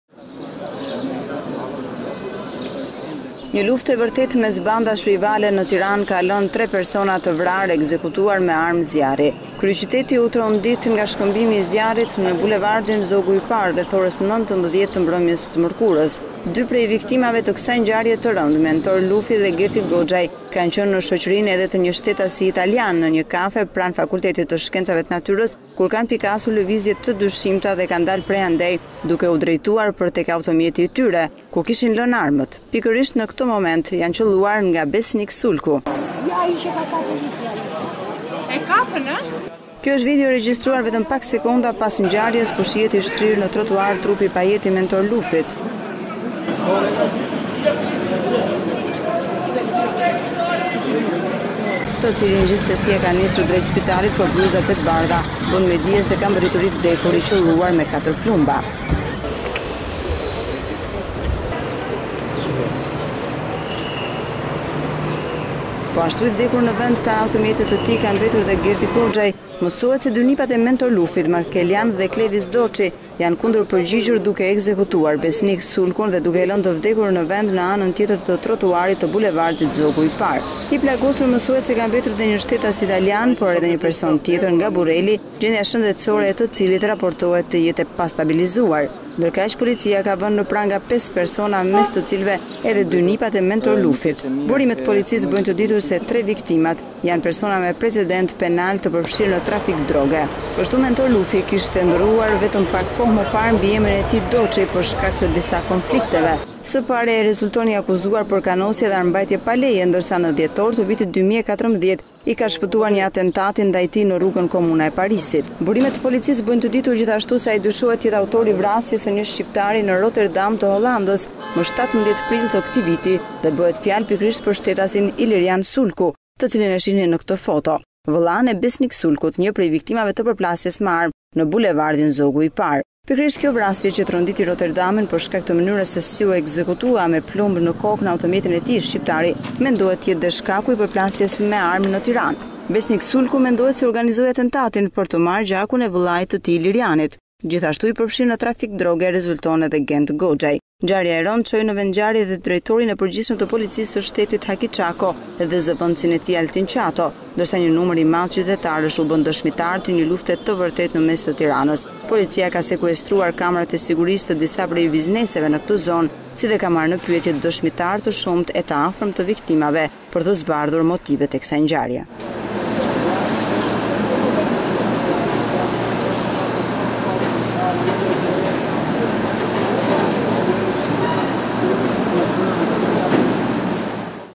Tiranë, Përplasje me armë te bulevardi ‘Zogu i Parë’, vriten tre persona- Ora News- Lajmi i fundit-
Një luftë e vërtetë mes bandash rivale në Tiranë ka lënë tre persona të vrarë, ekzekutuar me armë zjarri. Kryeqyteti u trondit nga disa të shtëna në bulevardin Zogu i Parë rreth orës 19 00 të 27 majit 2015
2 prej viktimave të kësaj ngjarje të rëndë, Mentor Lufi dhe Gerti Goxhaj kanë qënë në shoqërinë edhe të një shtetasi italian në një kafe pranë Fakultetit të Shkencave të Natyrës kur kanë pikasur lëvizje të dyshimta dhe kanë dalë prej andej duke u drejtuar për tek automjeti i tyre, ku kishin lënë armët. Pikërisht në këtë moment janë qëlluar nga Besnik Sulku.